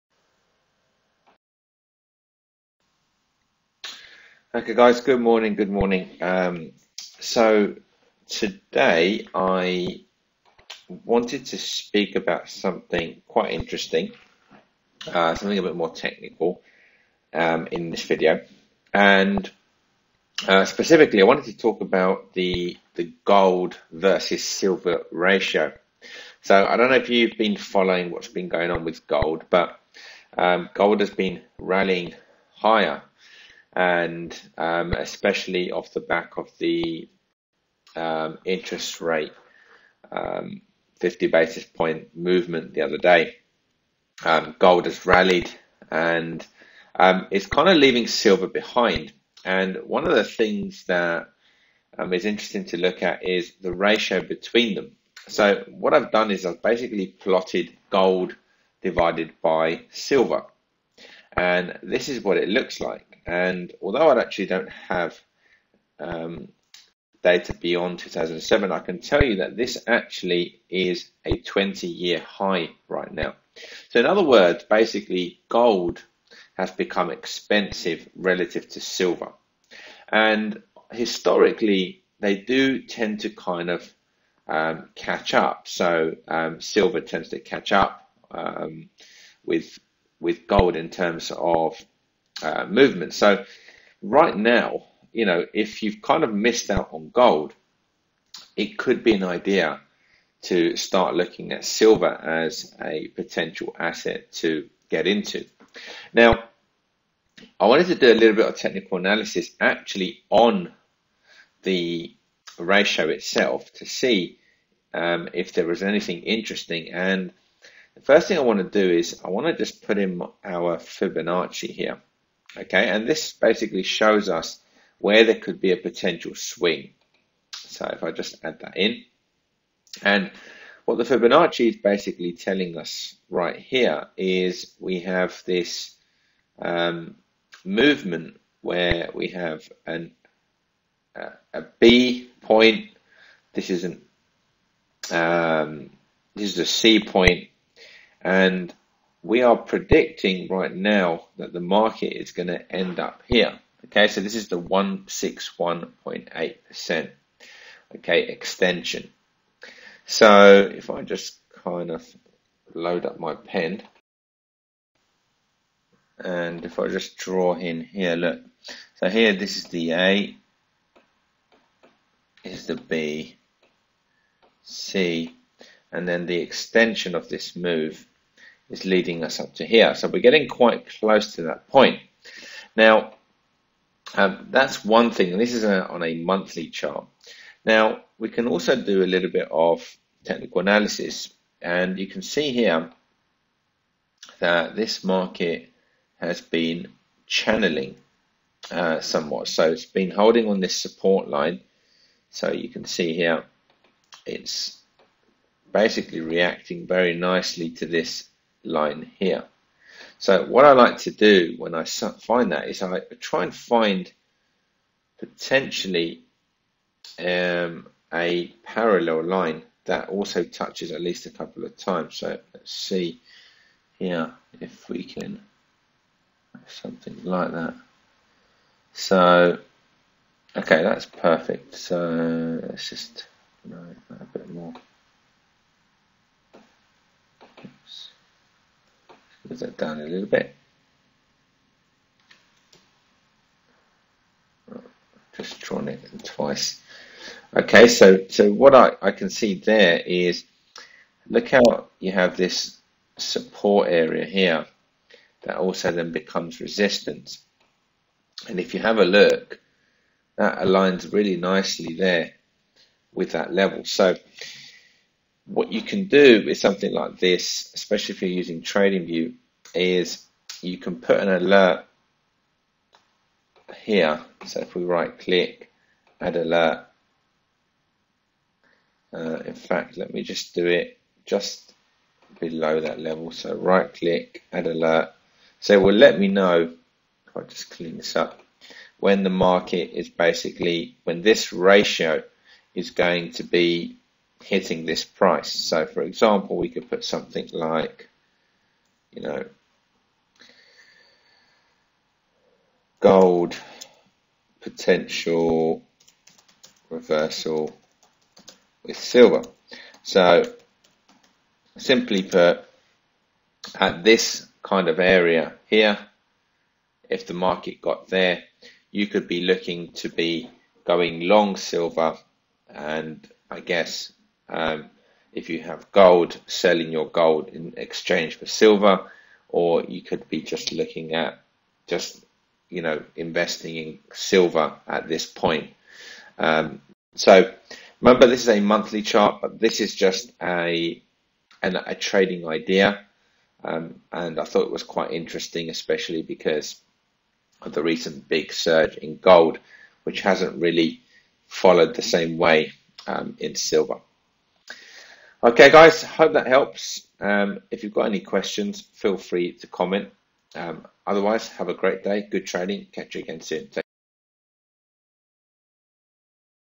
Gold-Silver Ratio at 20 Yr High
The recent fed rate cut by 50 bp has led to a continuation of the Gold rally, but Silver has been left behind. Today we analyse the gold/silver ratio and discuss when it could be time to switch to silver. Have a great day everyone